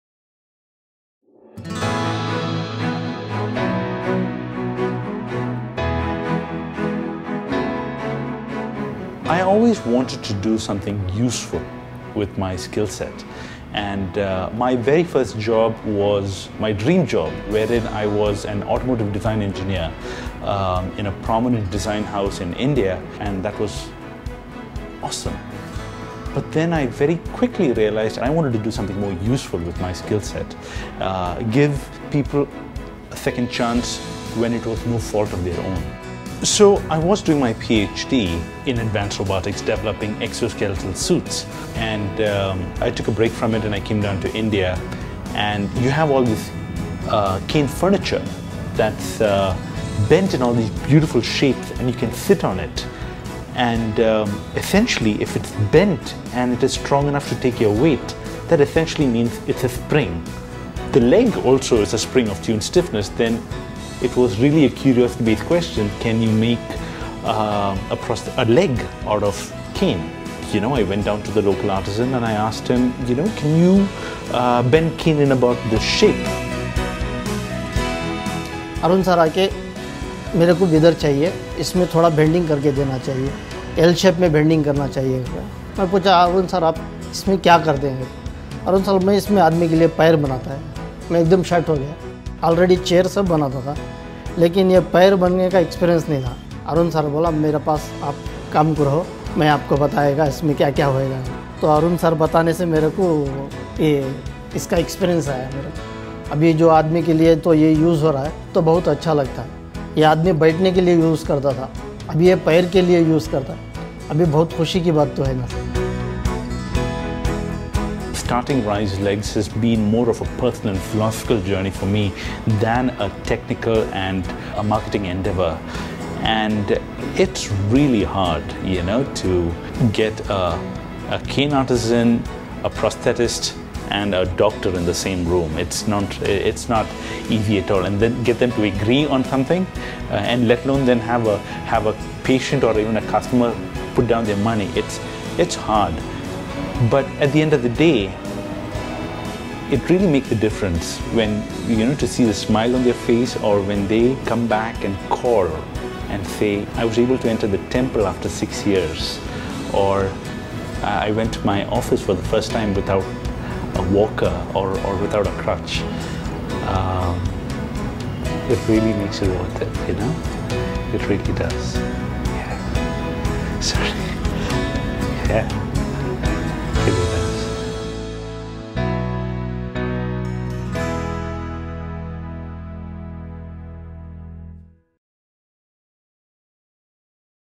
StilliRise - Jossbox x Rise Legs
So far, Rise Legs has received an immense amount of support for the Cybathlon. Bangalore’s Play Arena has donated their space to the Rise Legs team in order to conduct training on an obstacle course which has been custom built to mirror the one at the Cybathlon. Potential Health Development and Invictus Prime, two Bangalore-based companies, have contributed extensive hours by creating and executing tailor made strength, conditioning and nutrition programs for the athletes.

Rise Legs is taking this opportunity to build and grow a community of para-athletes and coaches in India, while also supporting organizations in the field.

Support Rise Legs by logging on to riselegs.jossbox.com